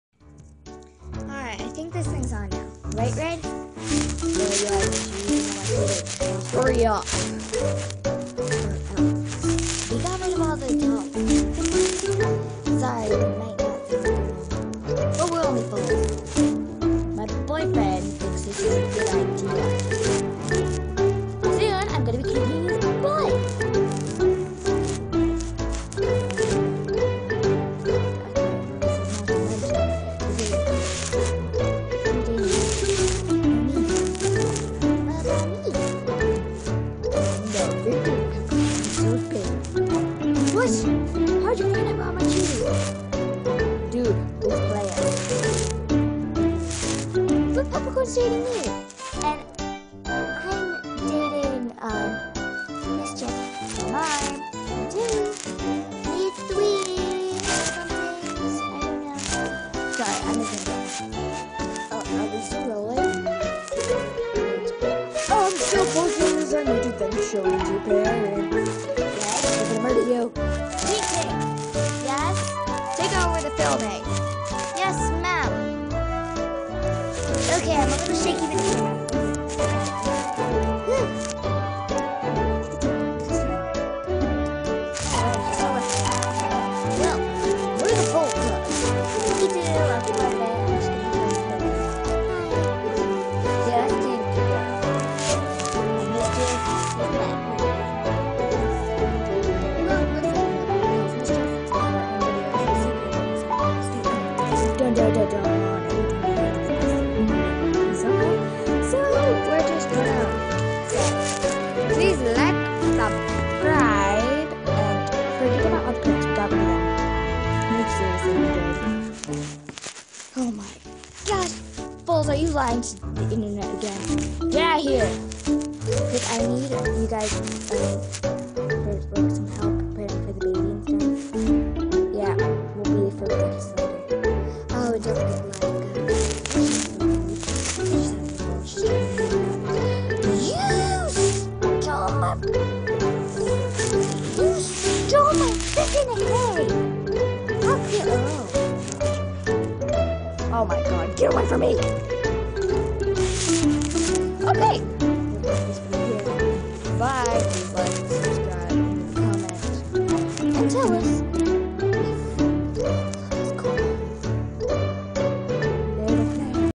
The foal club
Lol tiny foals think they can take on the world against full grown horses. They are so stupid.